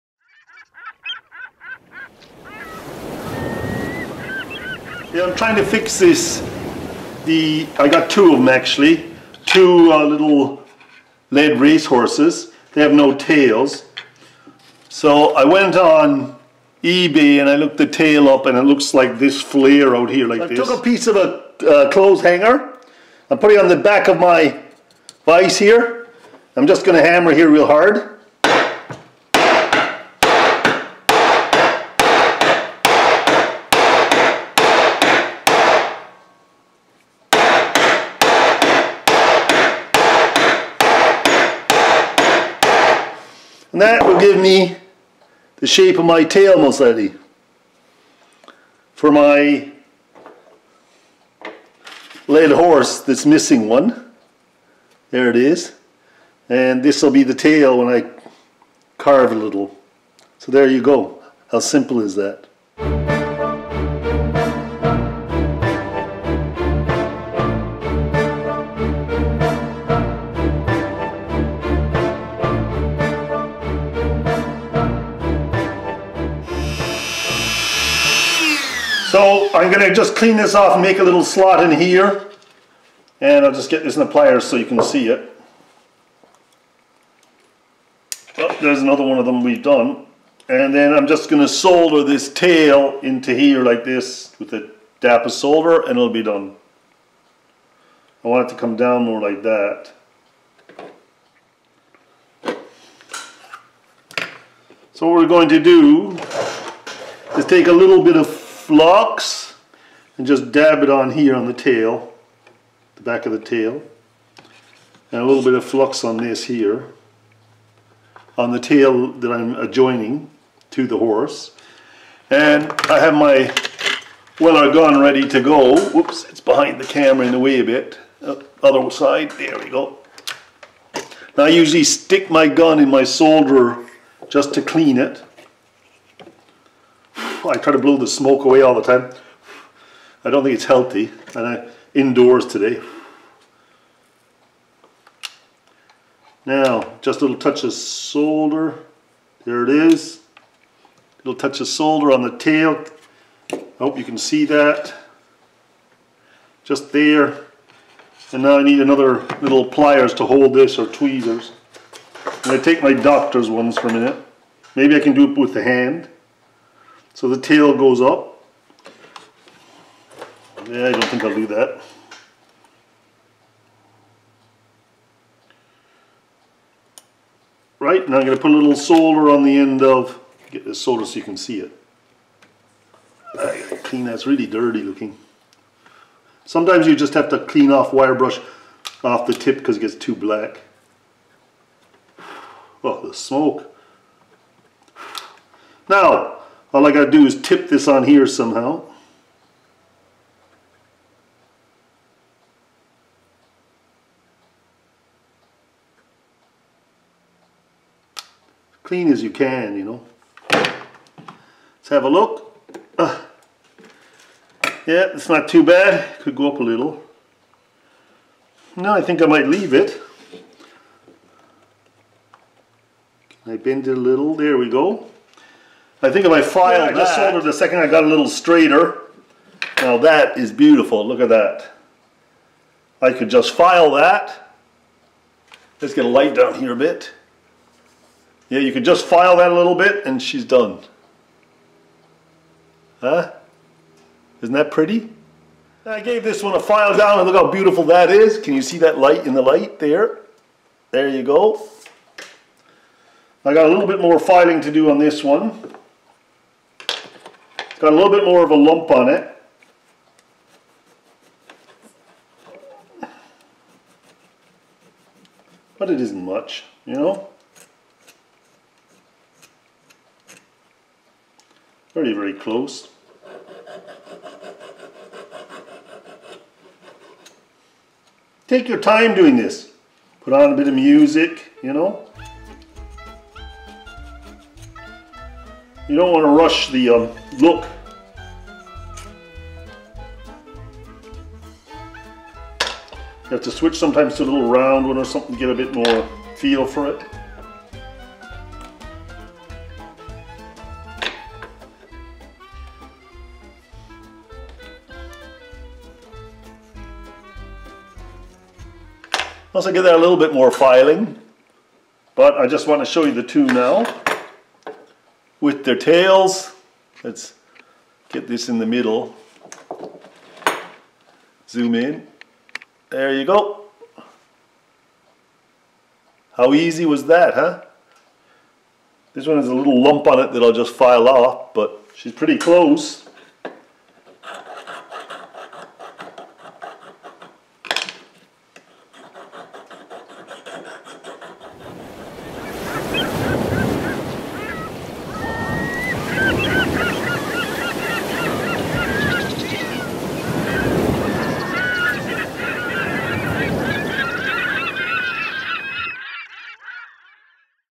HOW TO RESTORE LEAD FIGURES 2
Lead Race Horse Restoration: In this video I restore lead figures with a soldering gun.
I picked up two damaged lead race horses in London. Now I'm working on restoring them. The tails were missing so I researched them on eBay and copied the shape of the tail.